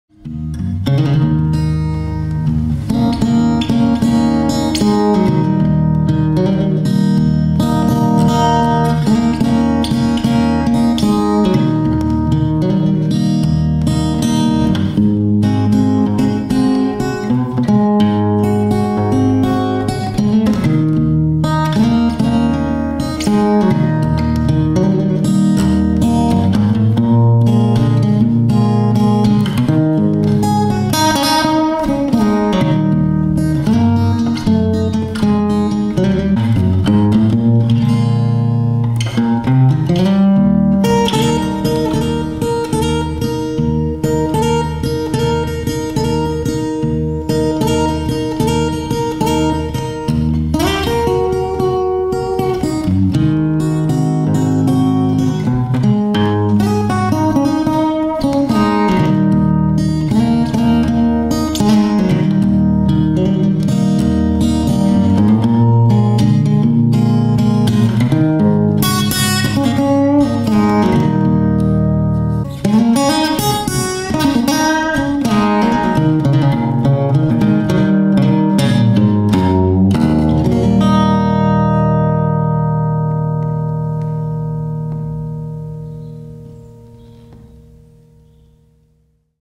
Acoustic Blues - 1940 Gibson L-30
Acoustic blues played on a 1940 Gibson L-30 archtop.